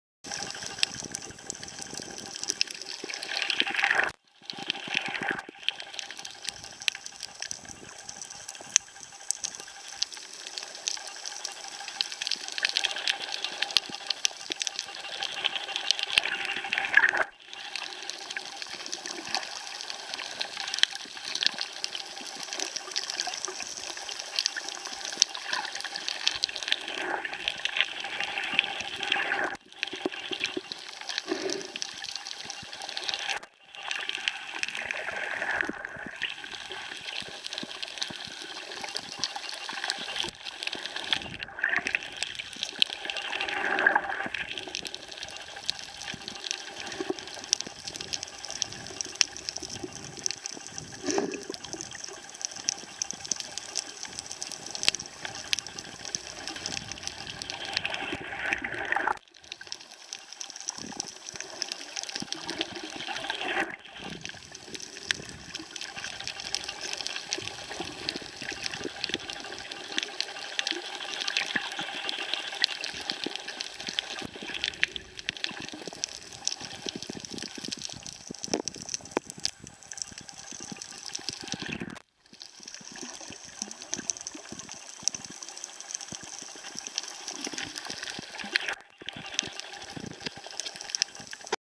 Biyadhoo Snorkeling in the Maldives
snorkeling along the house reef wall